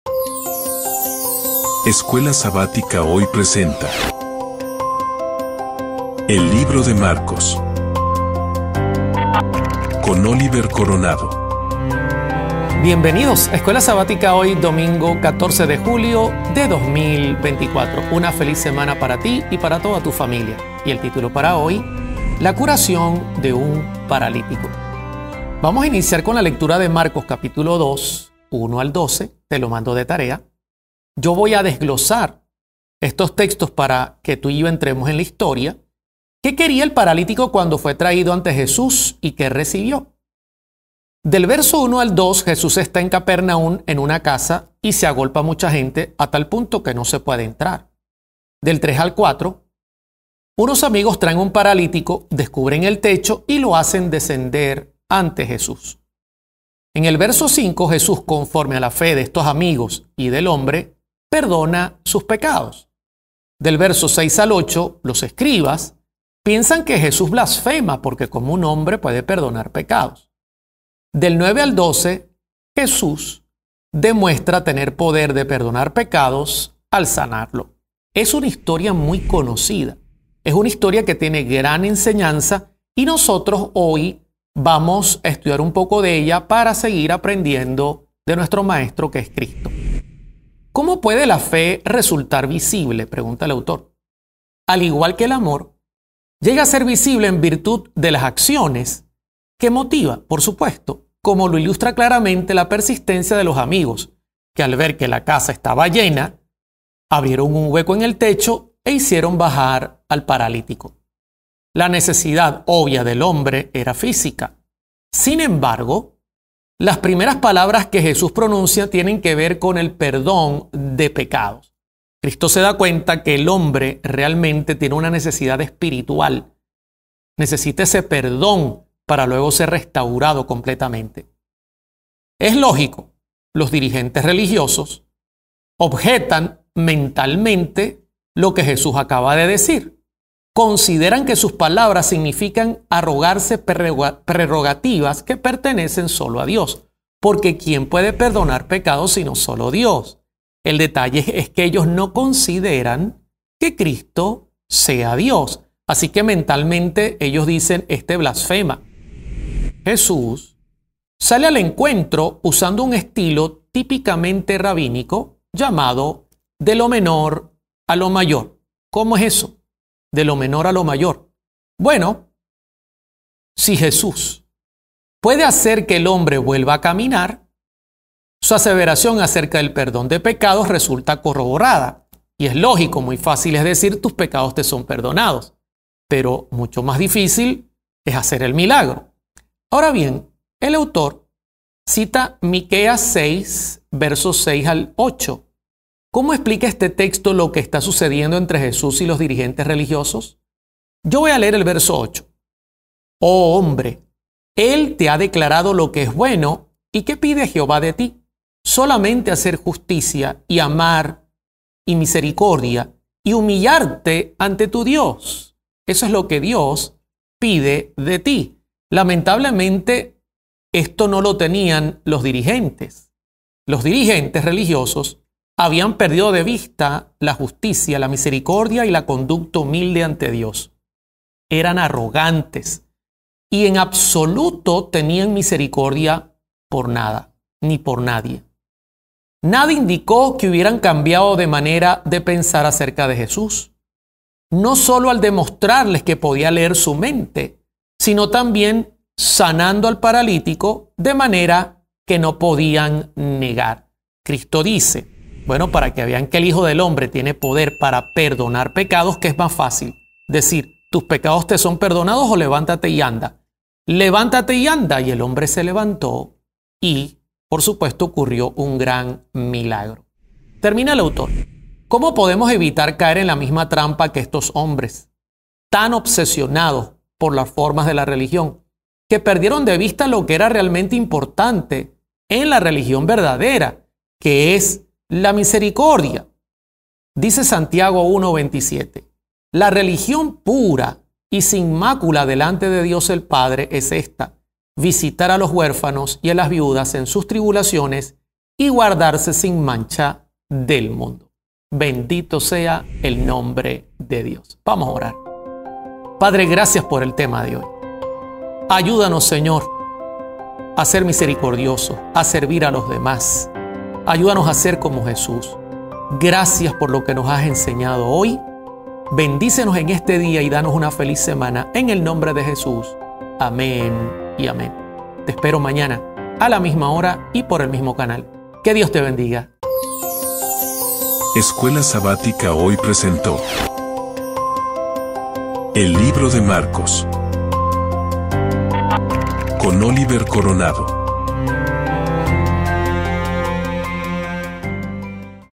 ESCUELA SABÁTICA HOY | 14 DE JULIO DE 2024
¡Feliz Semana¡ ¡Gracias por estudiar con Nosotros!

📌 LA CURACIÓN DE UN PARALÍTICO  - LECCIÓN 3

DOMINGO 14 de Julio de 2024:

Escuela Sabática HOY - con Oliver Coronado PRESENTA:

"El Libro de Marcos..." Autor: Thomas R. Shepherd.

Un estudio de 5 minutos, sencillo, directo, profundo, y muy innovador.

📌 DESCARGA LA APP QUE USAMOS: Adventech

📌 GRACIAS por compartir con todos tus hermanos y amigos, este estudio de la Lección de Escuela Sabática...

CORONA STUDIO TEAM